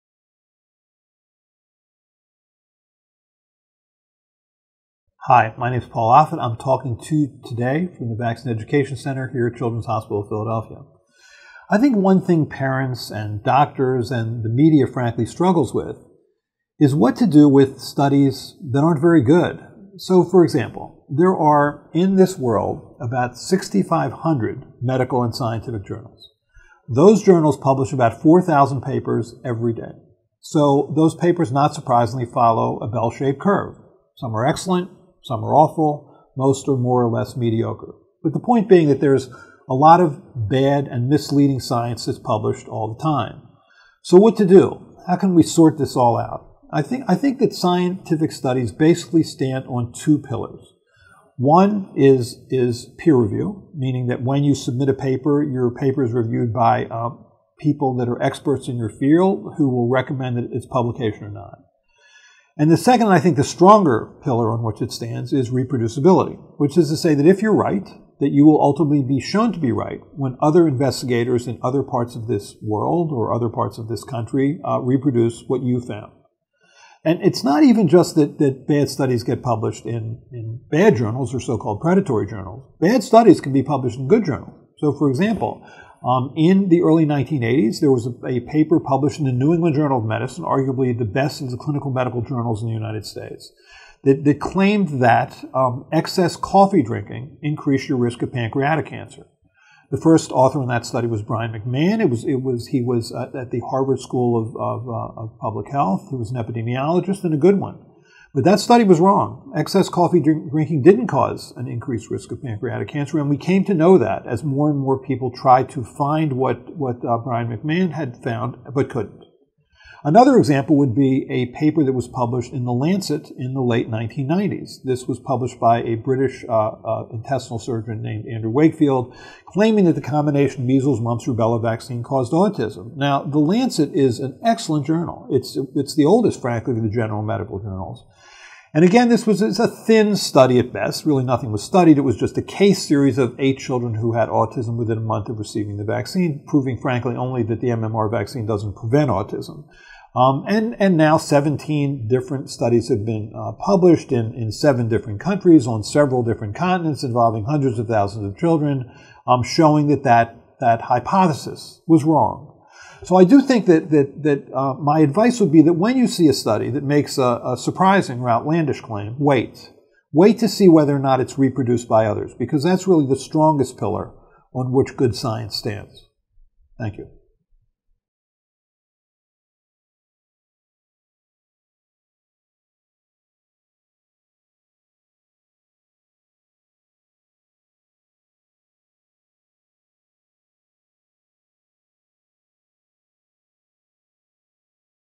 Parents PACK Science Made Easy: Bad Studies: Yes, They Exist. How Can You Identify Them?
Dr. Offit talks about how to evaluate scientific studies using two important pillars of science — peer review and reproducibility.